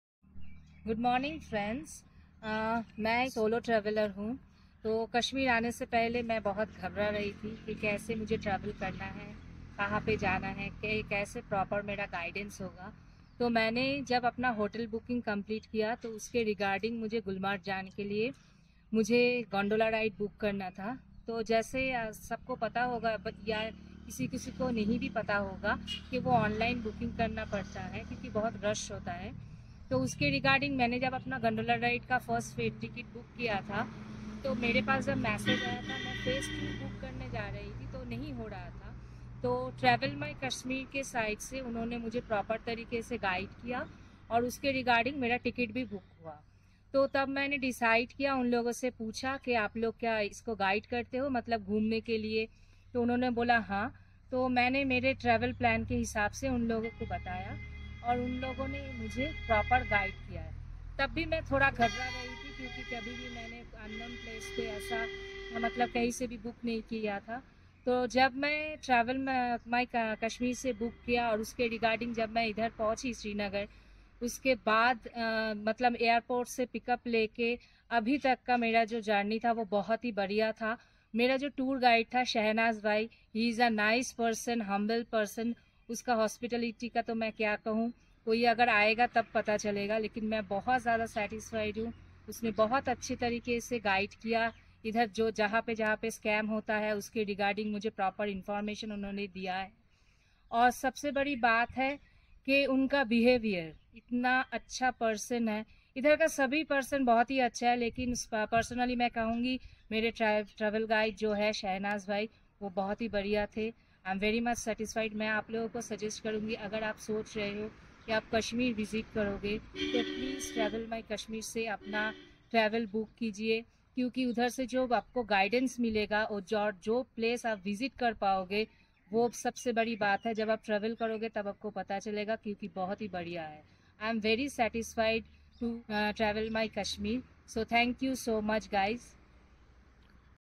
Is it Safe to Travel in Kashmir? | Solo Travelling Women in Kashmir | Best Cab Service in Kashmir
This lovely lady (Dona) who travelled SOLO being a women , assures all the upcoming tourists that the Kashmir vicinity is Safe for Travel.